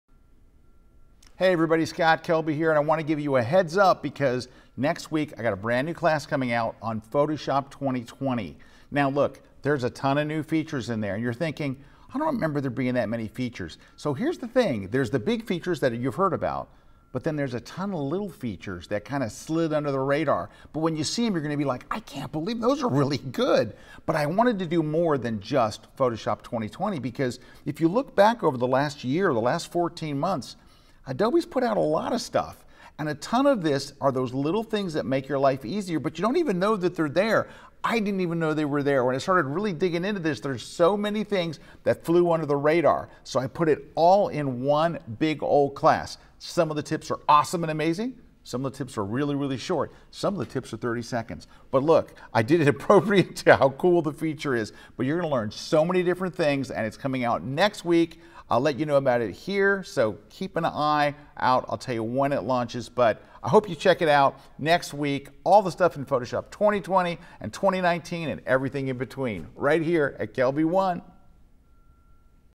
Heads up on my new Photoshop 2020 Online Course
It's not just what's in the most recent 2020 update — this is all that stuff that you've missed in the last year — all those little updates, tweaks, new features and enhancements that Adobe added that don't really get any press, or even mentioned. It's lots of those hidden gems from the last 14 months or so. The class is coming next week (the week of Nov 18th, 2019)  at KelbyOne.com